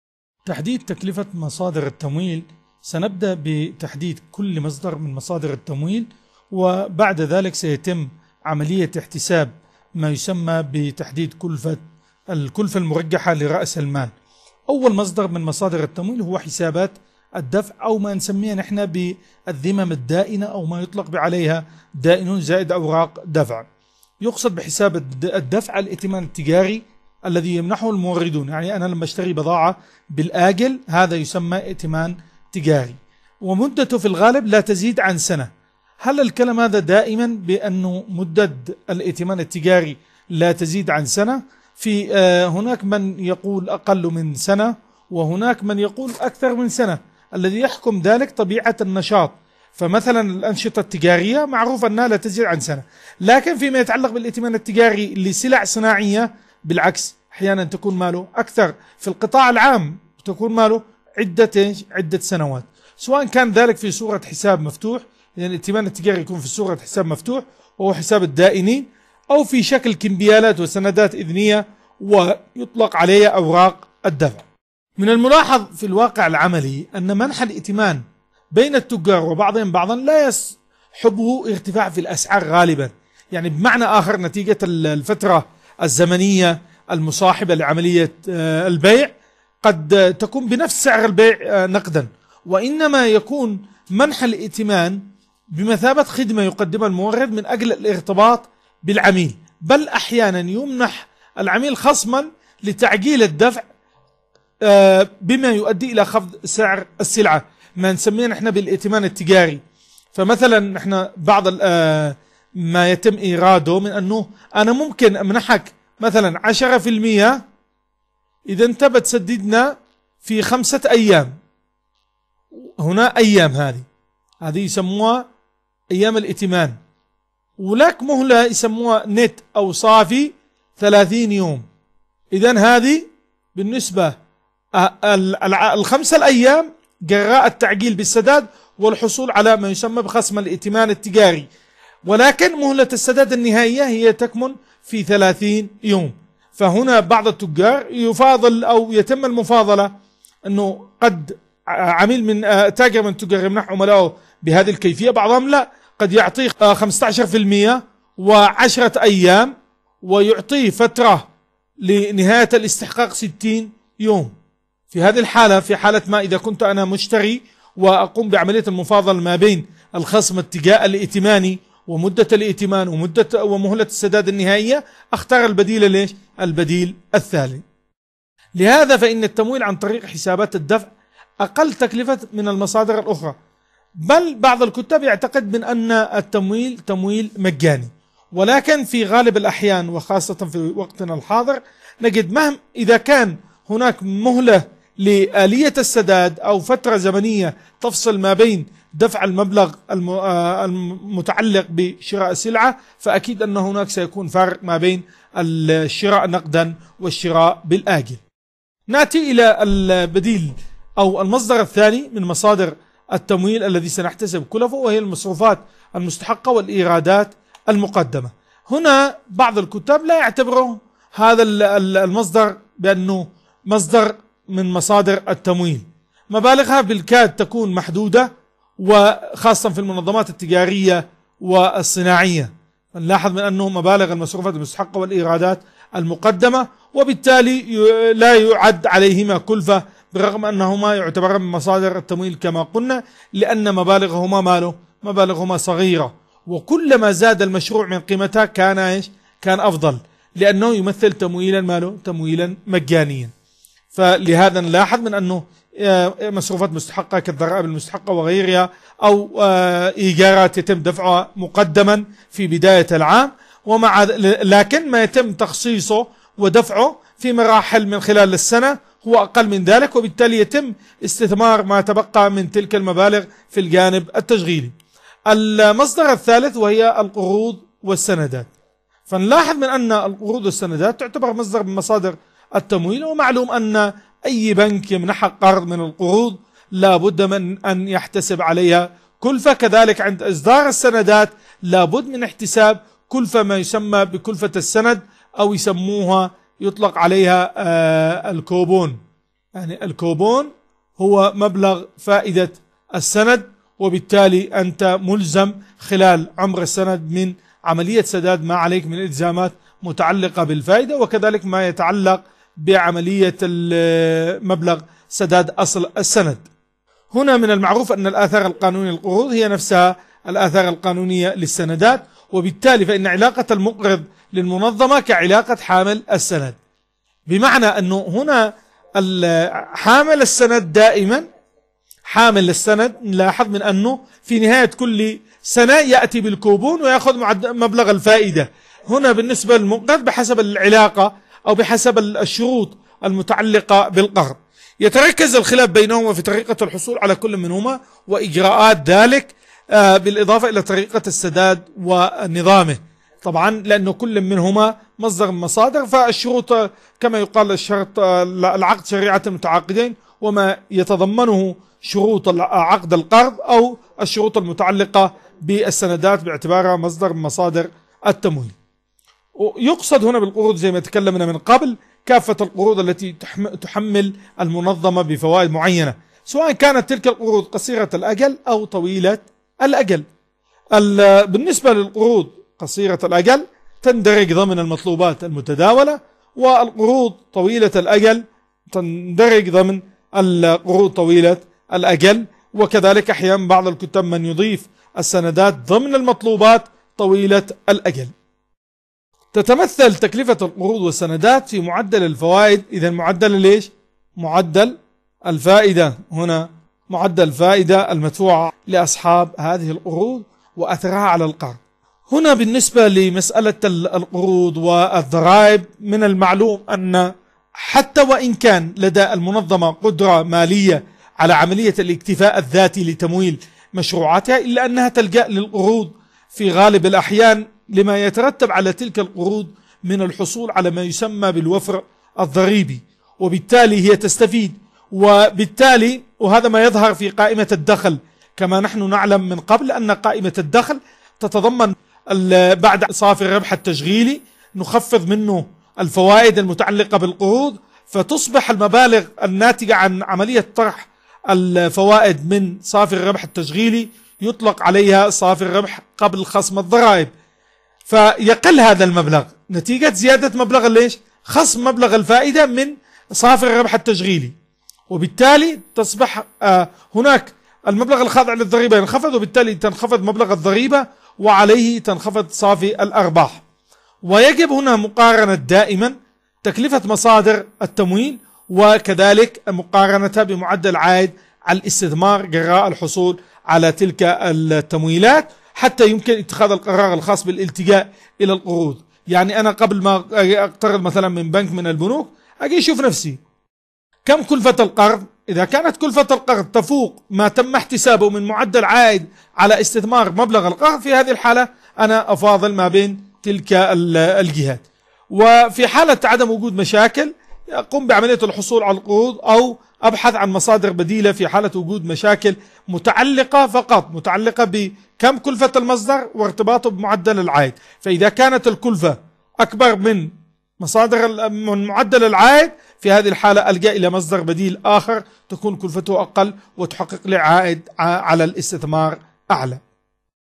الوحدة الرابعة (تحديد تكلفة كل مصدر من مصادر التمويل) - مقرر الادارة المالية
مقرر الادارة المالية
مقدم من عمادة التعليم الالكتروني والتعليم المفتوح - جامعة العلوم والتكنولوجيا - اليمن